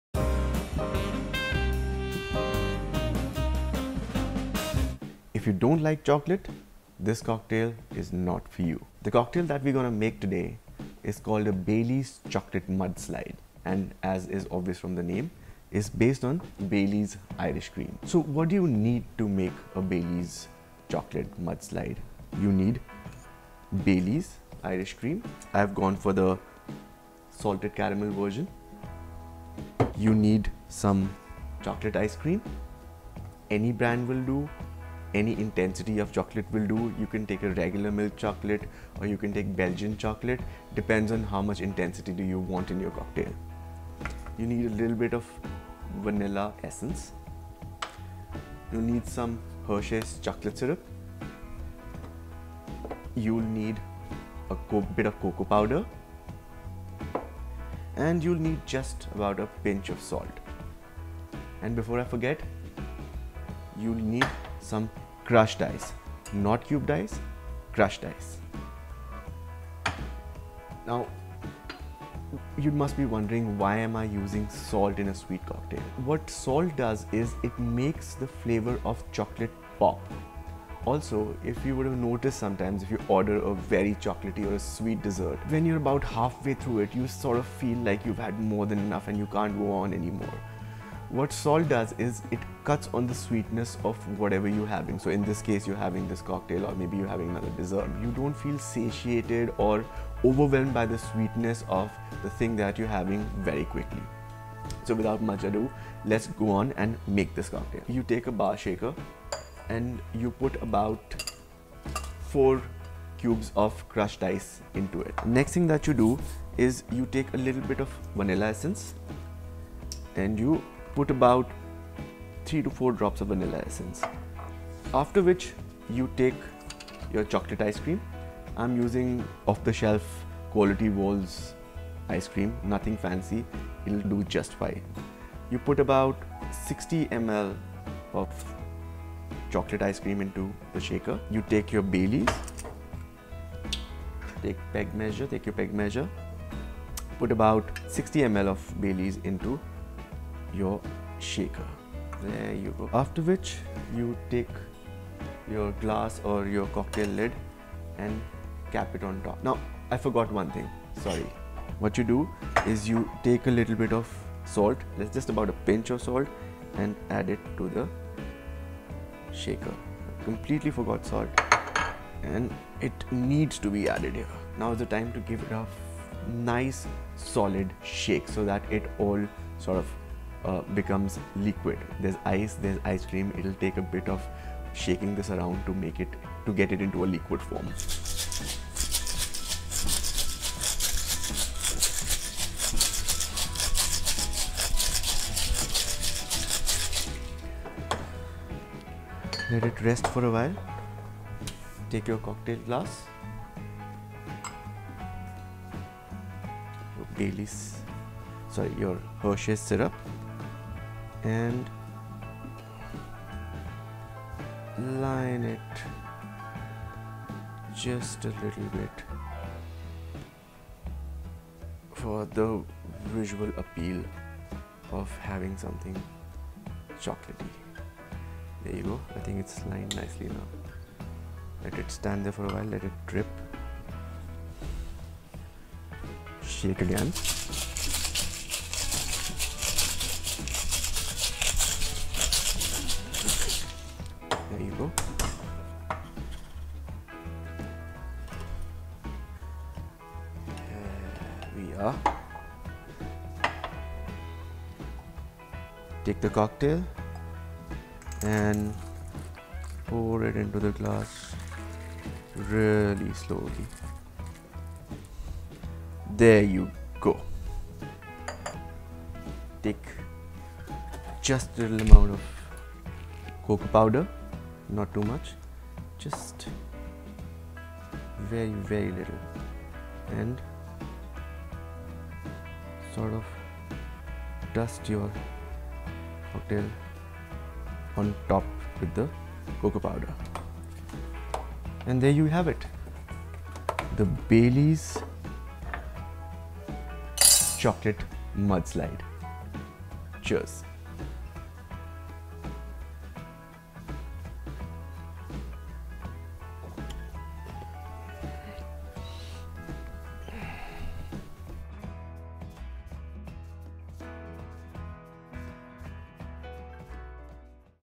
Baileys Chocolate Mudslide: Indulge in the Creamiest, Dreamiest Dessert Cocktail
A super delicious cocktail made of Baileys Irish Cream and chocolate ice cream.
This uses the salted caramel version of Baileys Irish Cream, but feel free to use regular Baileys as well!

All footage shot by me for Le Manual Experience